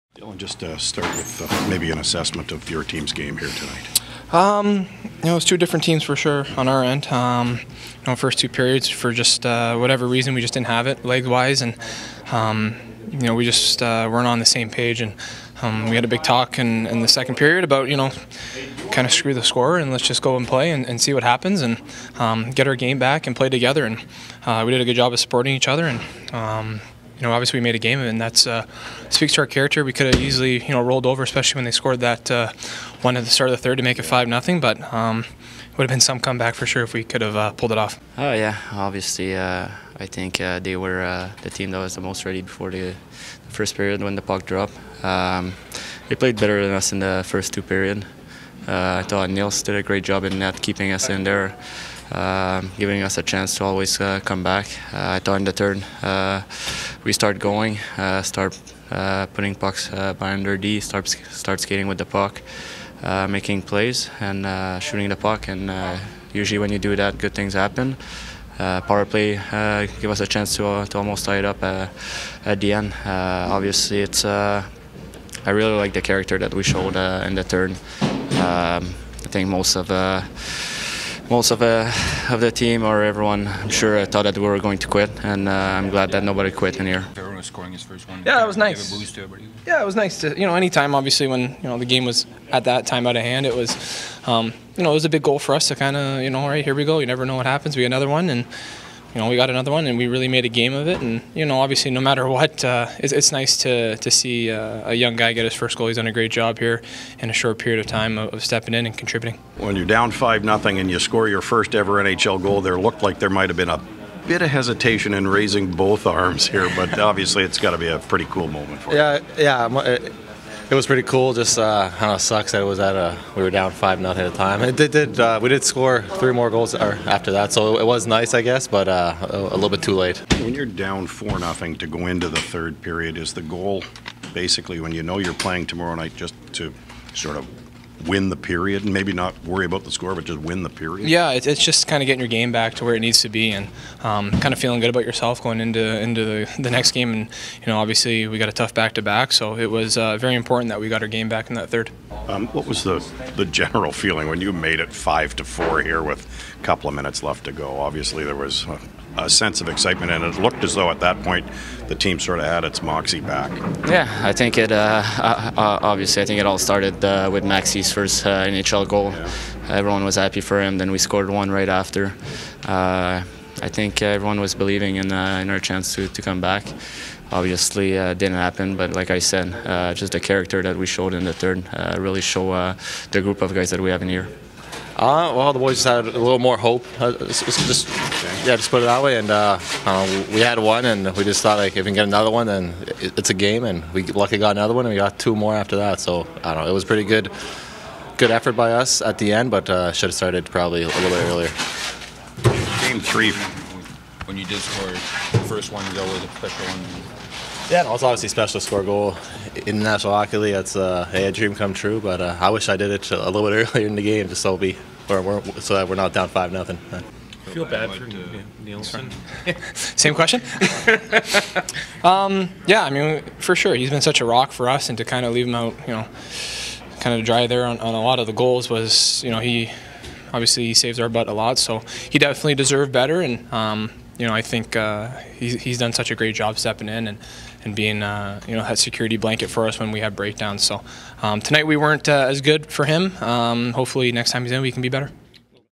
Sens vs. Canucks - Players Post-game
“We did a good job supporting each other and we made a game of it and it speaks to our character.”

Sens defenceman Dylan DeMelo speaks to the media following tonight’s 7-4 loss to the Vancouver Canucks at the Rogers Arena. Here’s your Molson Canadian post-game wrap-up.

Photo by Jeff Vinnick/NHLI via Getty Images